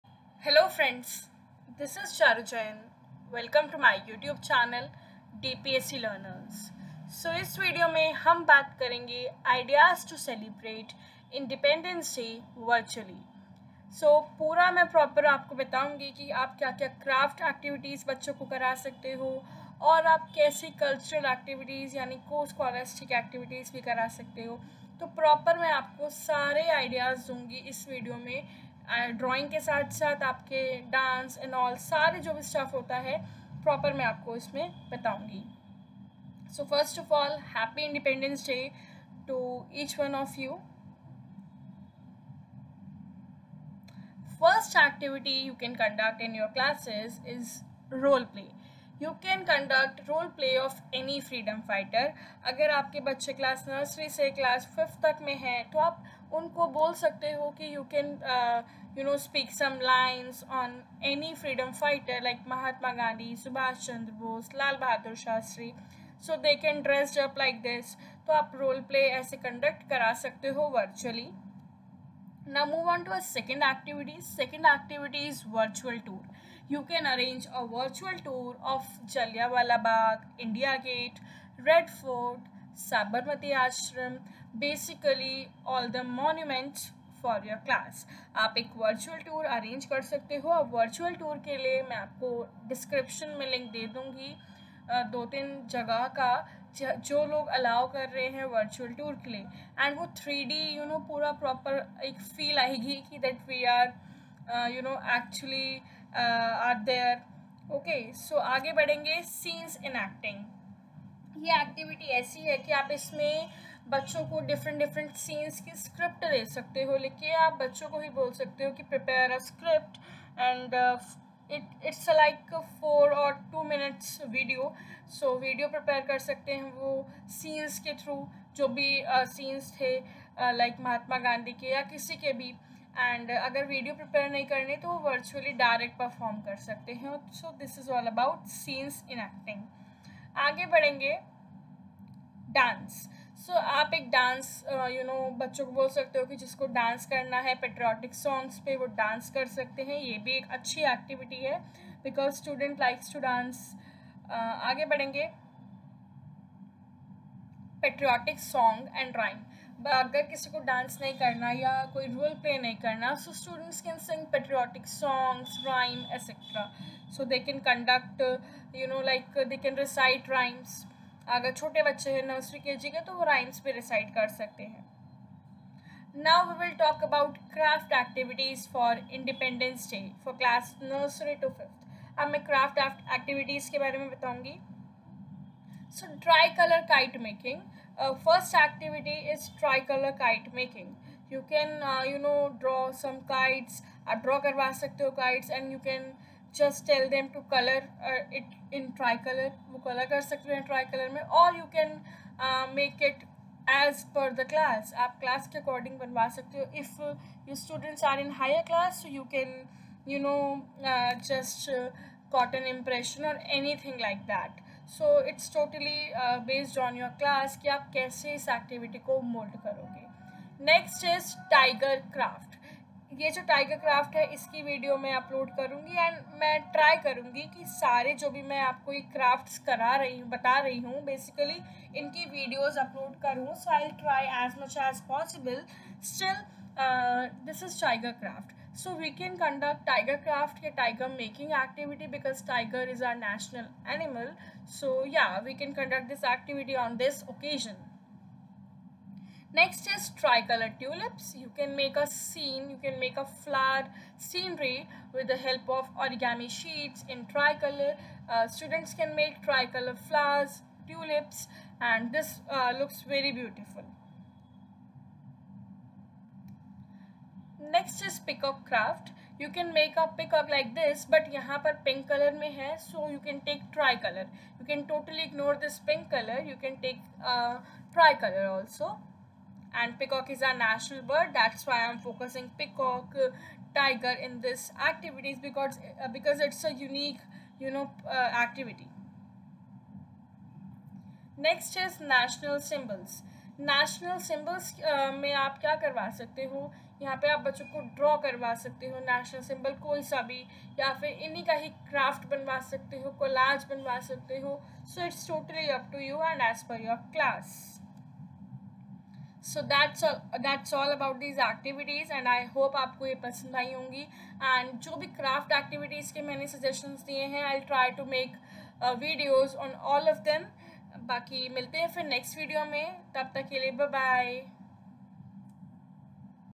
Independence Day Activities | Ideas To Celebrate Independence Day Virtually | Independence Day Craft
This video gives you the information about how you can celebrate Independence day virtually.

The link of the playlist- Gender,diversity and discrimination